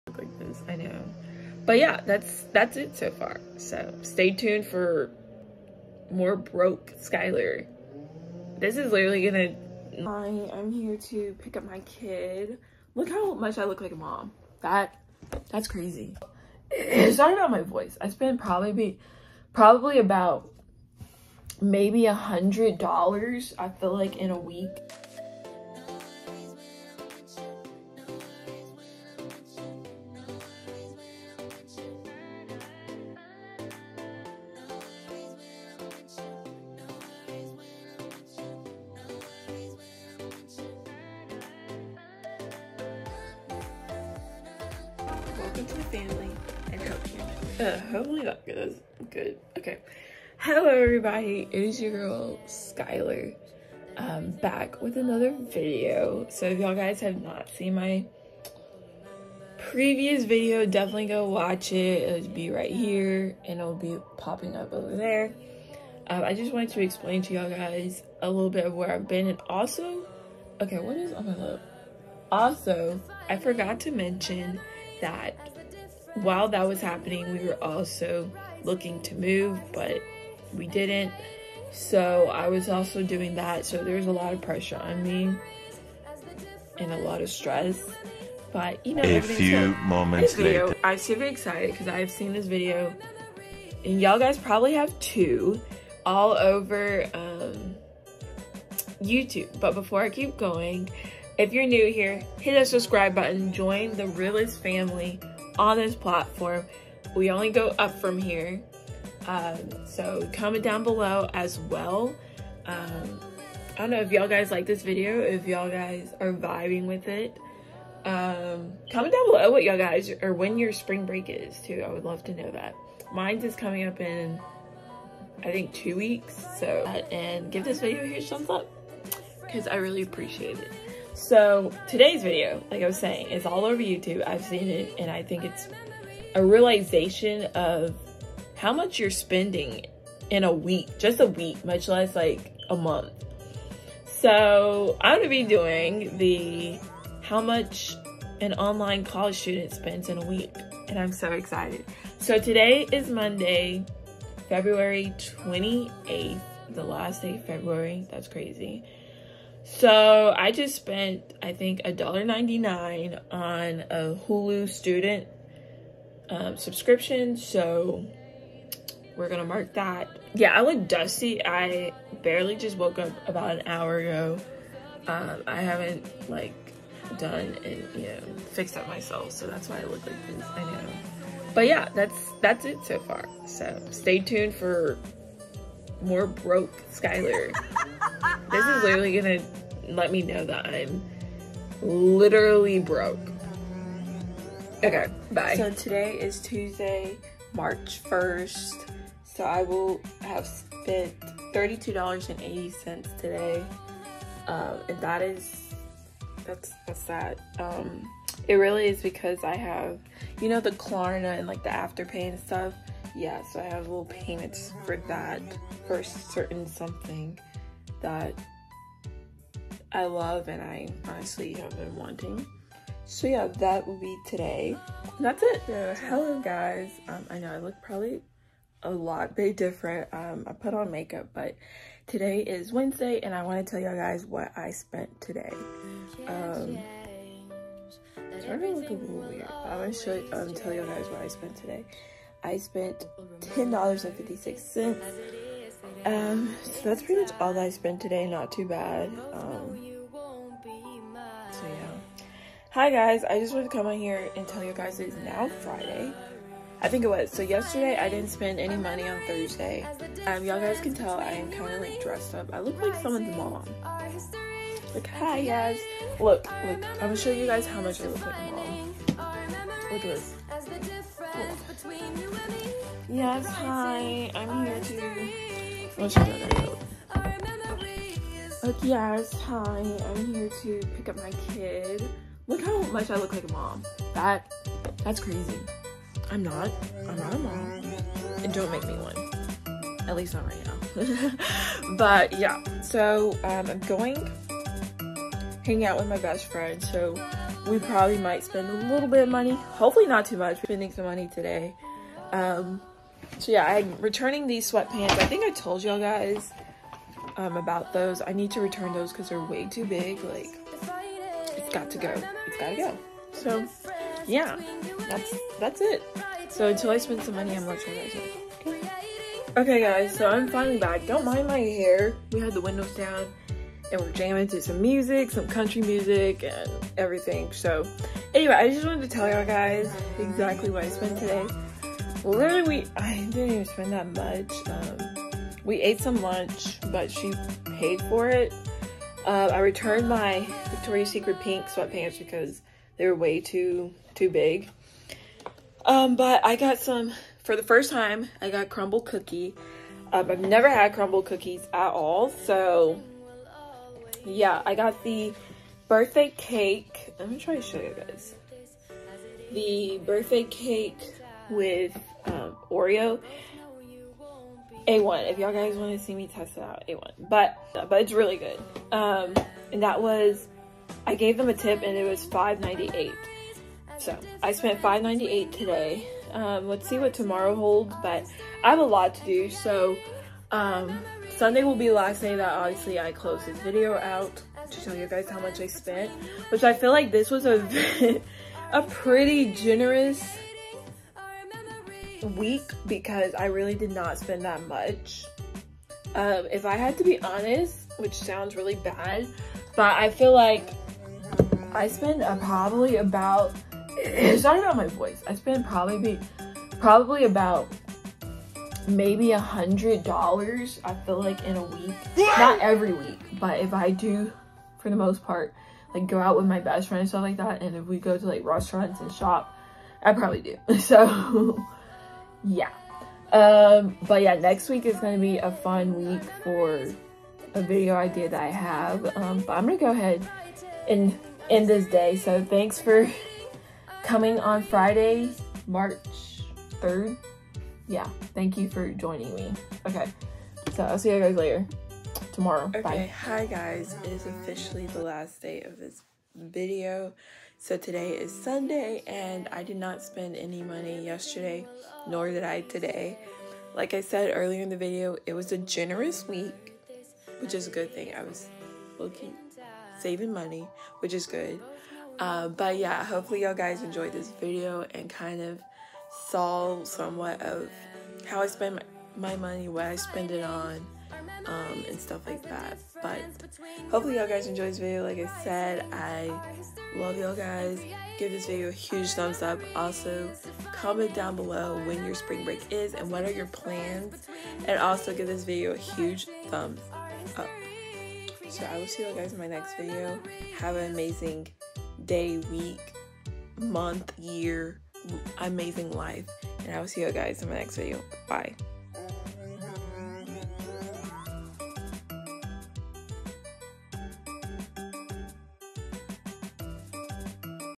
HOW MUCH I SPEND IN A WEEK (as an online college student)
This video is a realization somewhat of how much I spend as an online college student but it was not that bad if I say so. When you start taking note of what you spend in a week it gets REAL!!! Hope you guys enjoy.

Love you all. Hope you are having an amazing and blessed day. Remember stay strong.
New videos coming!

COMMENT: How much do you think you spend in a week?